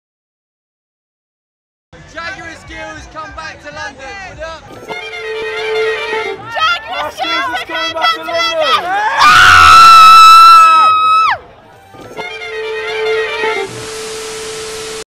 short Instagram video made for Jaguar Skills Return to London city.
when the ninja was heading back to the city to take over! i made this little Instagram just to highlight a few peoples excitement. Good times i say.